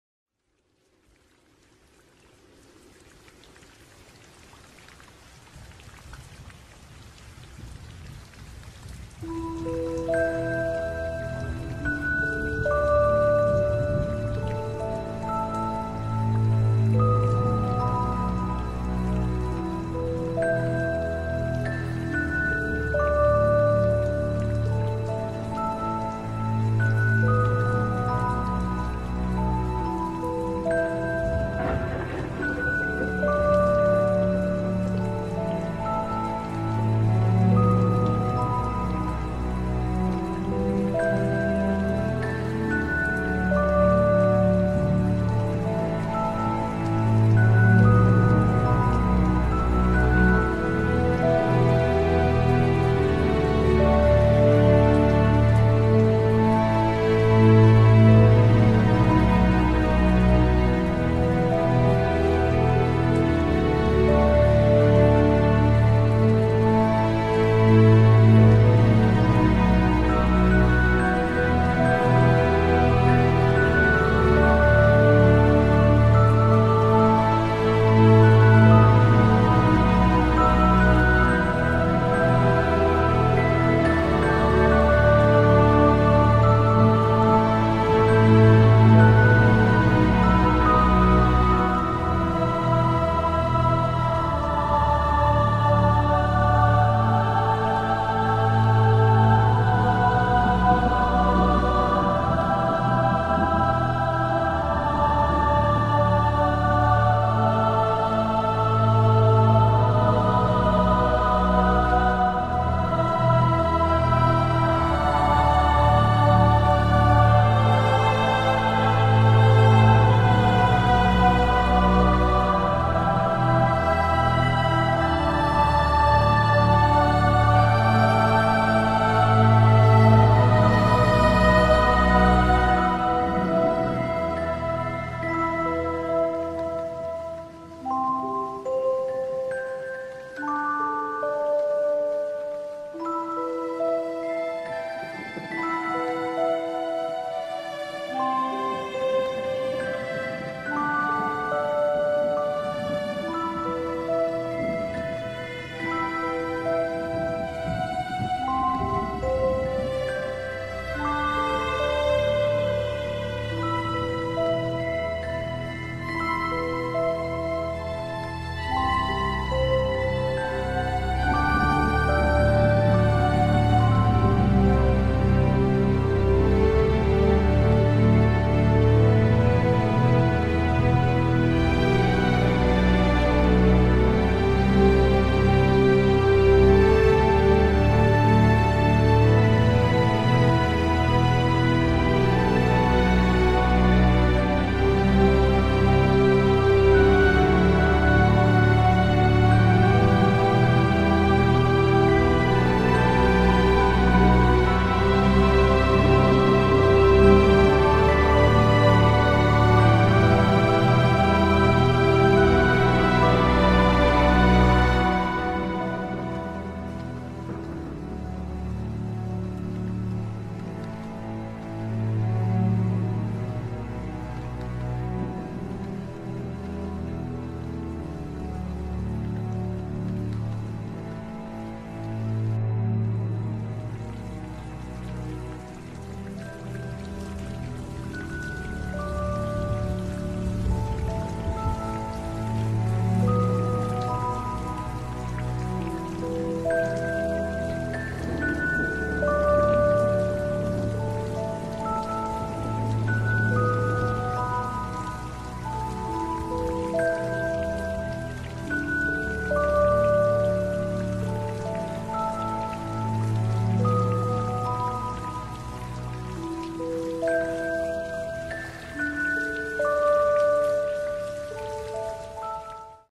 Paintings / Digital Art / Fractals / Sterling / by Sara Sund Berlin
These fractals i created with software Sterling. A special THANK YOU to Adrian von Ziegler that i may use your music while present my Digital ArT.